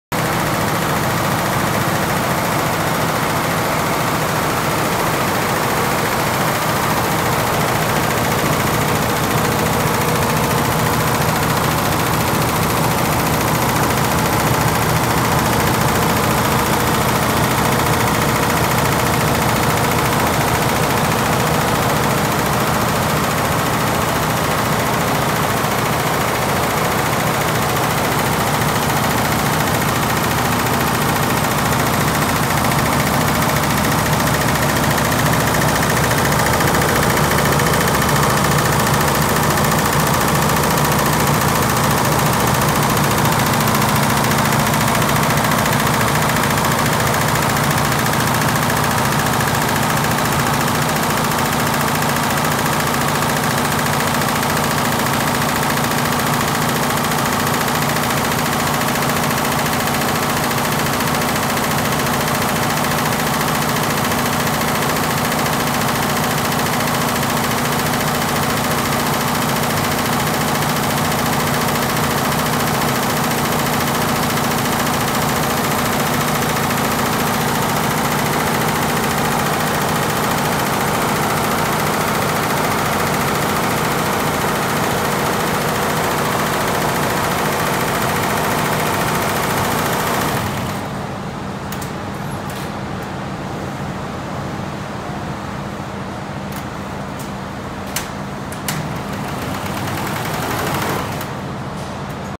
113 HEADS/110 HEADS SPACE, LEJIA SUPER MULTI HIGH SPEED BEST EMBROIDERY MACHINE
In this video we can find Lejia embroidery machine can customized more than one hundred heads. Multi heads but run stable.